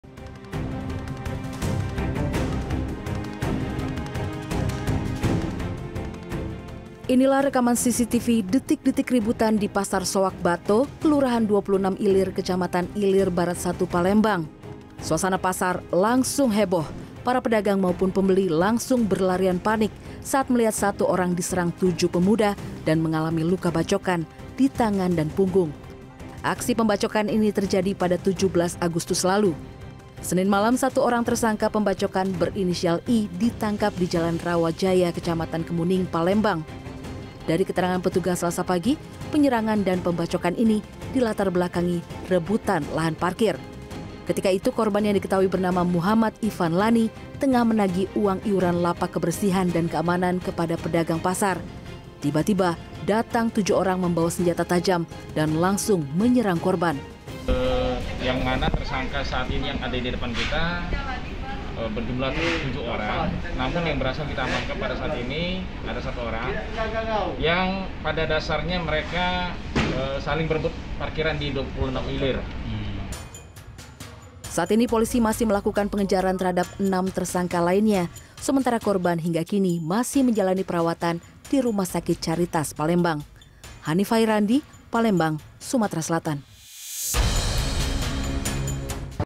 Pelaku Pembacokan di Pasar Ditangkap
Setelah sempat buron, satu dari 7 pelaku pembacokan di Palembang Sumatera Selatan, ditangkap petugas, selasa pagi. Aksi pembacokan ini sempat terekam oleh kamera pengawas.